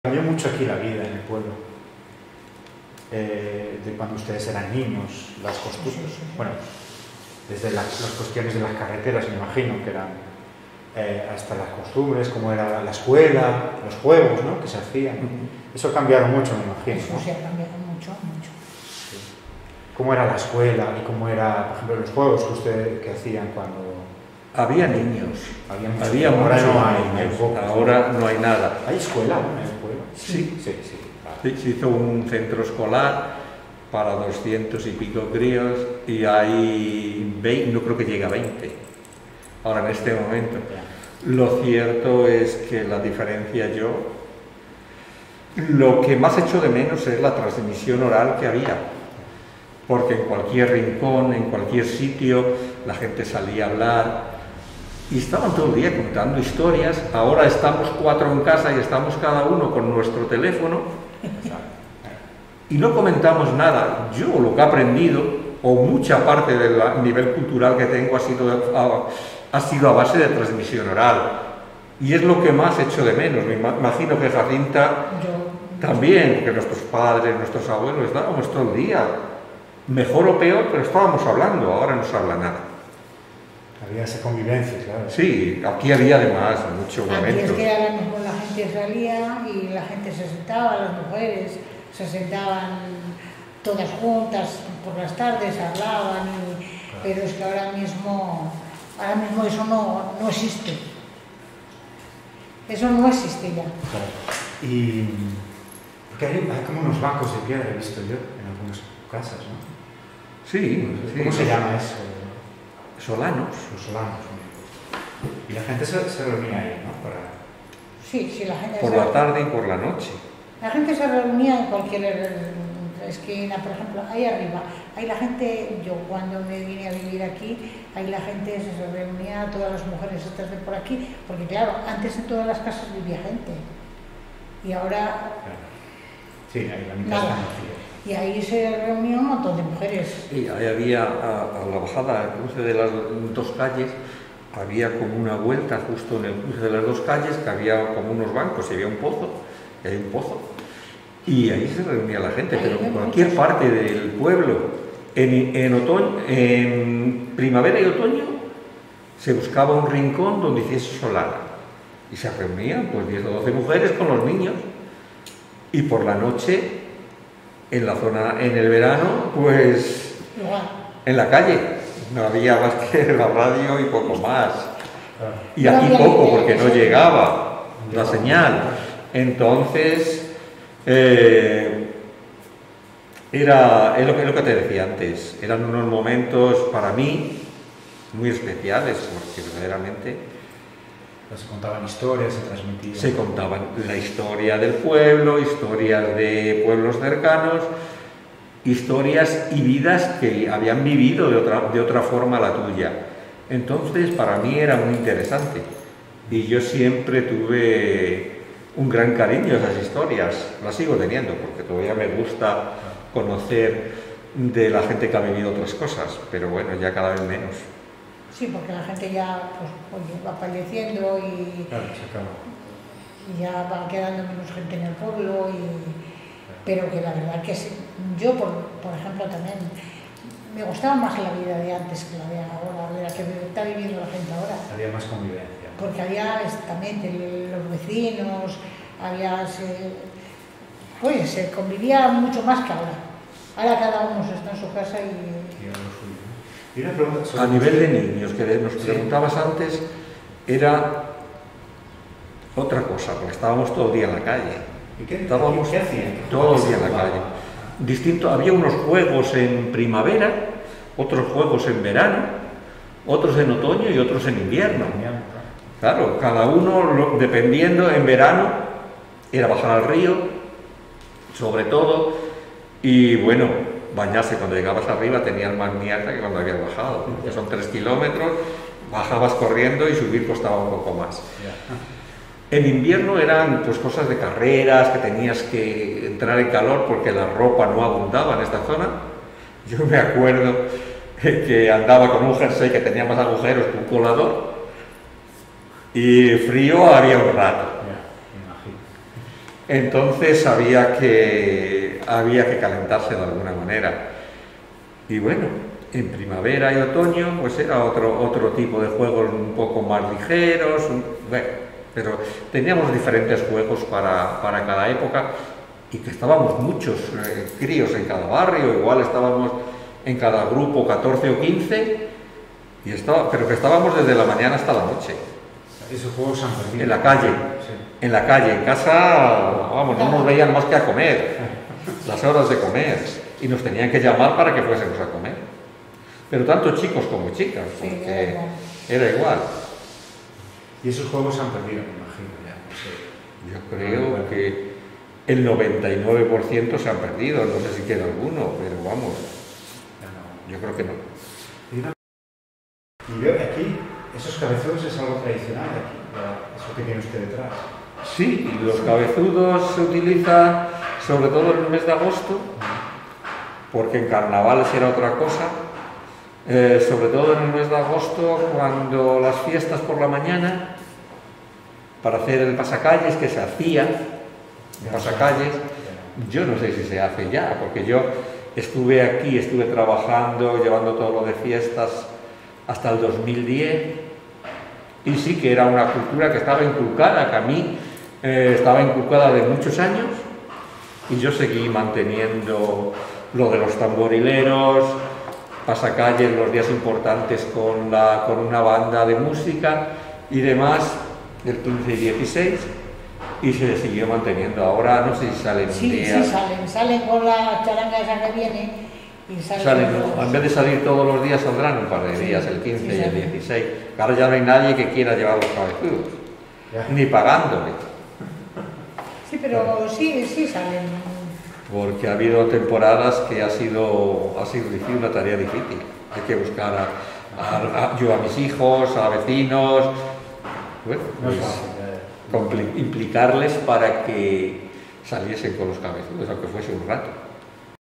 Villarino de los Aires 01. Sesión 03. La vida de antes
ESP. La principal diferencia es que antes había muchos niños; ahora deben quedar como una veintena. Otro cambio es que antes había mucha conversación en el pueblo, la gente estaba todo el día en las calles, conviviendo. Los juegos infantiles cambiaban según la época del año: ir al río, hacer carreras, etc. El formato tradicional de las fiestas. Los cabezudos.

PORT. A principal diferença é que costumava haver muitas crianças; agora devem restar cerca de vinte. Outra mudança é que costumava haver muita conversa na aldeia, as pessoas passavam o dia nas ruas, socializando. Os jogos das crianças mudavam consoante a época do ano: ir ao rio, correr corridas, etc. O formato tradicional das festas. Os cabeçudos.

ENG. The main difference is that there used to be a lot of children; now there must be about twenty left. Another change is that there used to be a lot of conversation in the village, people were in the streets all day, socialising. The children's games changed depending on the time of year: going to the river, running races, etc. The traditional format of the festivities. The big-heads.